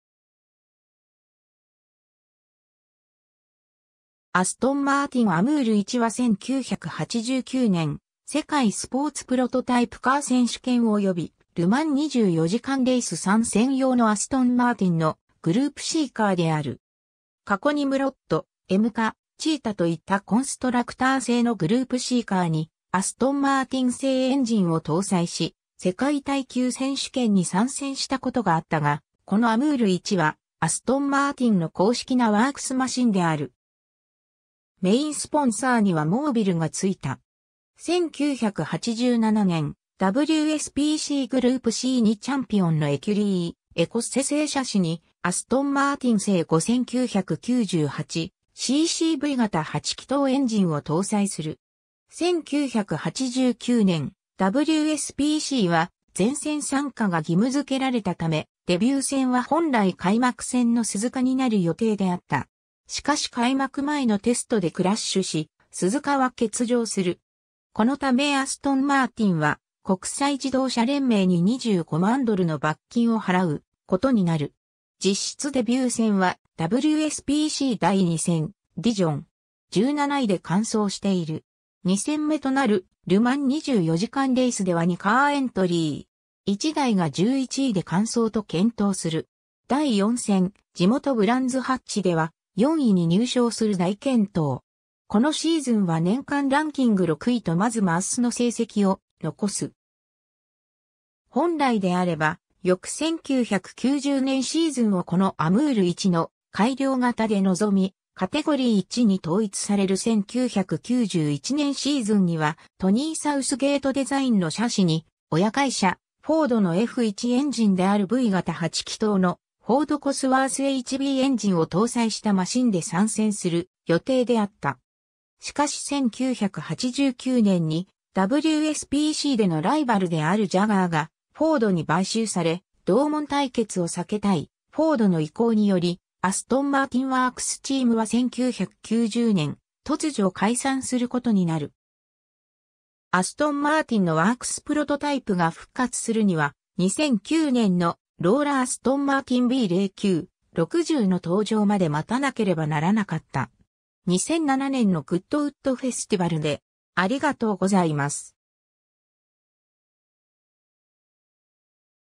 アストンマーティン・AMR1
アストンマーティン・AMR1は1989年世界スポーツプロトタイプカー選手権（WSPC）およびル・マン24時間レース参戦用のアストンマーティンのグループCカーである。
 過去ニムロッド、エムカ、チータと言ったコンストラクター製のグループCカーにアストンマーティン製エンジンを搭載し、世界耐久選手権（WEC）に参戦したことがあったが、このAMR1はアストンマーティンの公式なワークスマシンである。
メインスポンサーにはモービルが付いた。
 1987年WSPCグループC2チャンピオンのエキュリー・エコッセ製シャシにアストンマーティン製5,998 ccV型8気筒エンジンを搭載する。
 1989年WSPCは全戦参加が義務付けられたため、デビュー戦は本来開幕戦の鈴鹿になる予定であった。
しかし開幕前のテストでクラッシュし、鈴鹿は欠場する。
このためアストンマーティンは国際自動車連盟(FIA)に25万ドルの罰金を払うことになる。
 実質デビュー戦はWSPC第2戦ディジョン。
17位で完走している。
2戦目となるル・マン24時間レースでは2カーエントリー。
1台が11位で完走と健闘する。
第4戦地元ブランズ・ハッチでは、4位に入賞する大健闘。
このシーズンは年間ランキング6位とまずますの成績を残す。
 本来であれば、翌1990年シーズンをこのAMR1の改良型で臨み、カテゴリー1（3.5リットル自然吸気エンジン搭載車）に統一される1991年シーズンには、トニー・サウスゲートデザインのシャシに親会社フォードのF1エンジンであるV型8気筒のフォード・コスワース・HBエンジンを搭載したマシンで参戦する予定であった。
しかし1989年にWSPCでのライバルであるジャガーがフォードに買収され、同門対決を避けたいフォードの意向により、アストンマーティンワークスチームは1990年突如解散することになる。
 アストンマーティンのワークスプロトタイプが復活するには、2009年のローラ・アストンマーティン B09/60の登場まで待たなければならなかった。
 2007年のグッドウッド・フェスティバルで